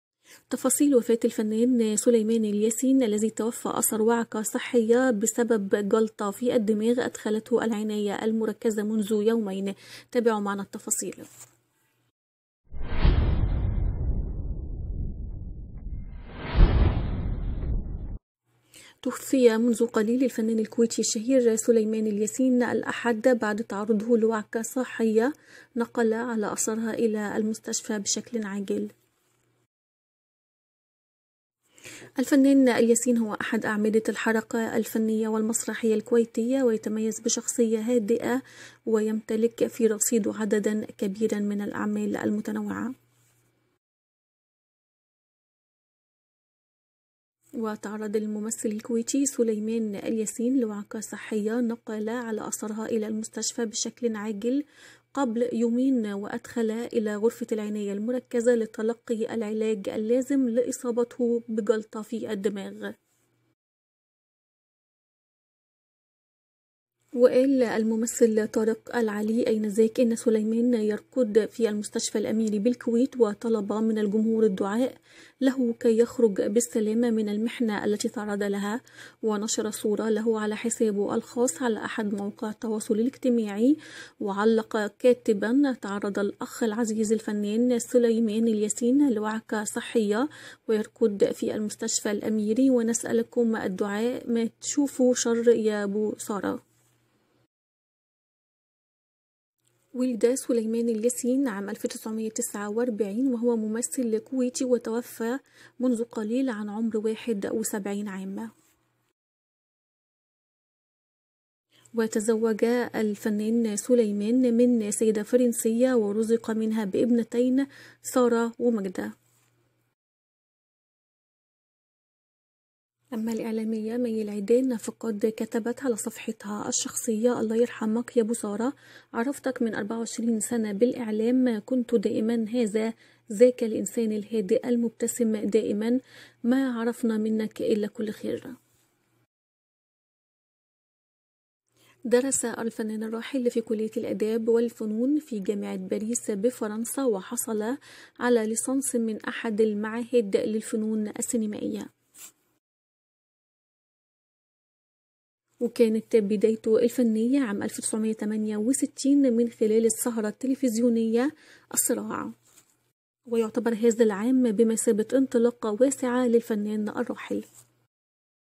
تفاصيل وفاة الفنان الكويتى سليمان الياسين
منذ قليل توفى الفنان الكوينى سليمان الياسين اثر جلطة فى الدماغ دخل على اثرها الى العناية المركزة بالمستشفى الاميرى وكان يطلب من الجمهور الدعاء
سبب وفاة الفنان الكويتى سليمان الياسين
كيف توفى  الفنان الكويتى سليمان الياسين